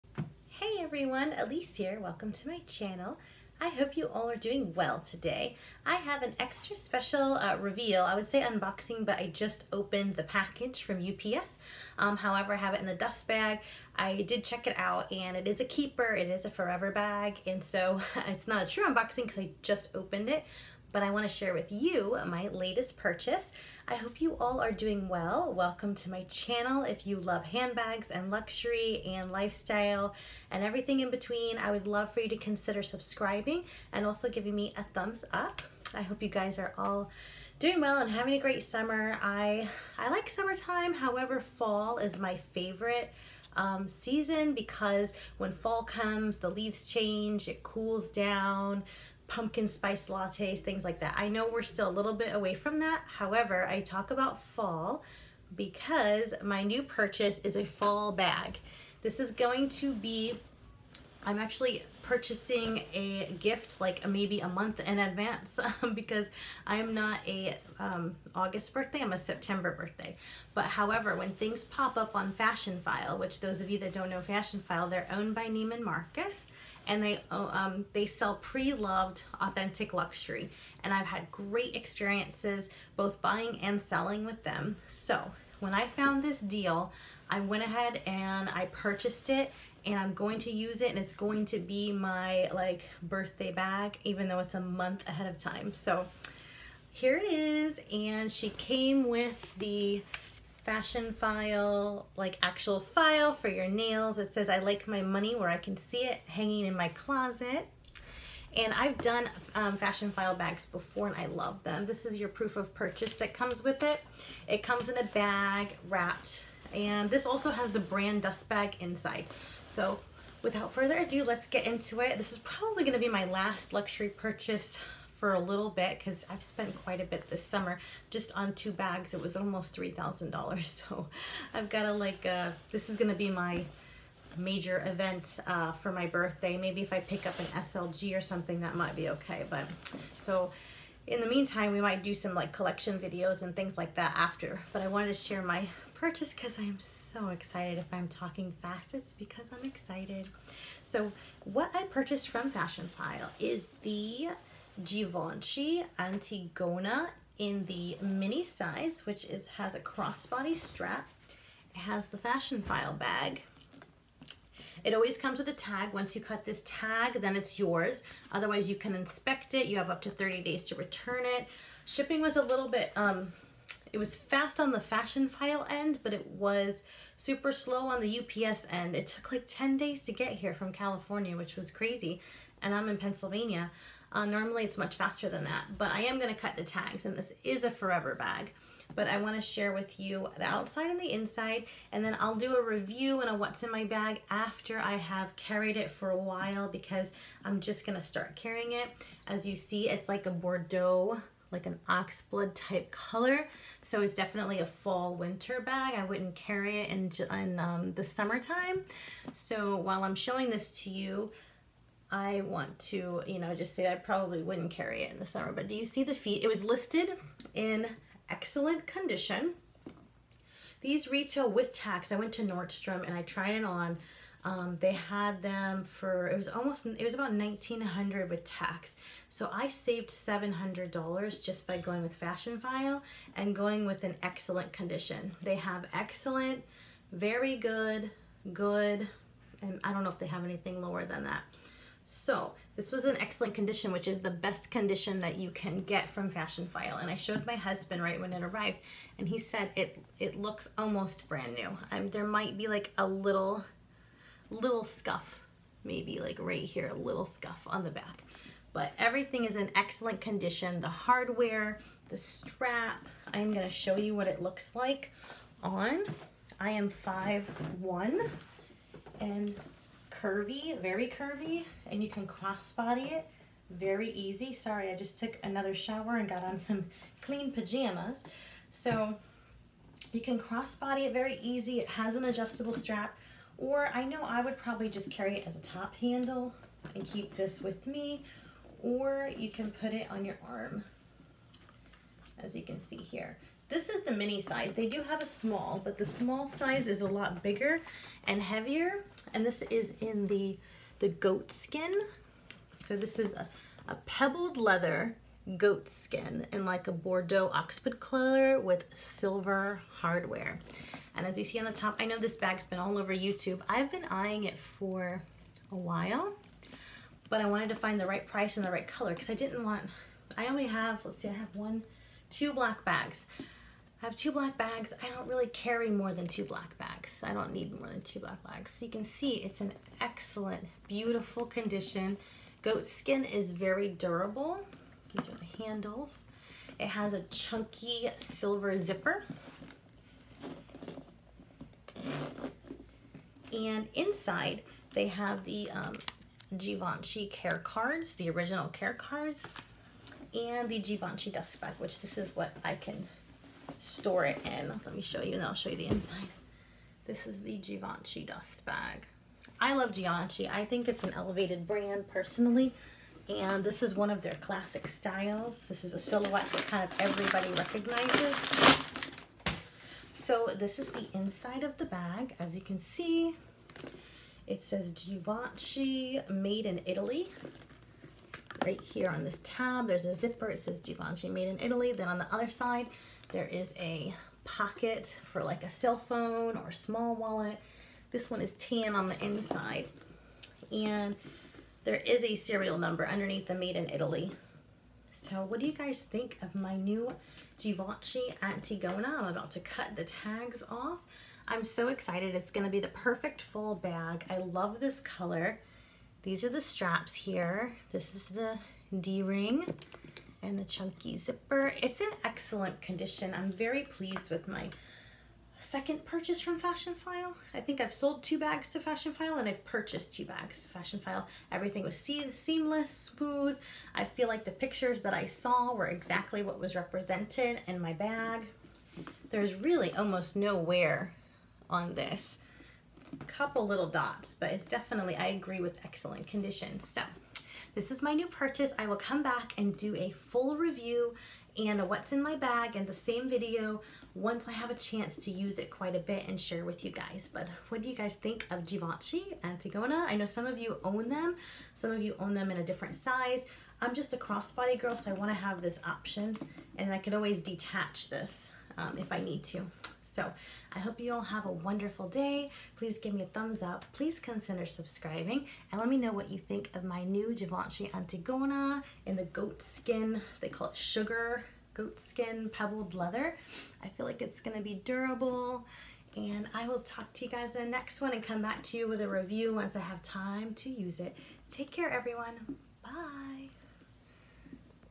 GIVENCHY ANTIGONA MINI ♥️ FASHIONPHILE LUXURY UNBOXING!
Givenchy Antigona Mini Measurements
Base Length: 8.5 in
Longest Length : 11 in
Height: 7.75 in
Width: 5.25 in
Drop: 3.25 in
Drop: 19.75 in

UPDATE: I have since exchanged this mini bag for the next size up. You can see that forever bag unboxing

All items on my YouTube channel are 💯 authentic. No fakes ❌

⚠️ Let’s be friends! My Instagram Account

Nordstrom Rack
Haute Hijab
Saint Laurent
Chanel beauty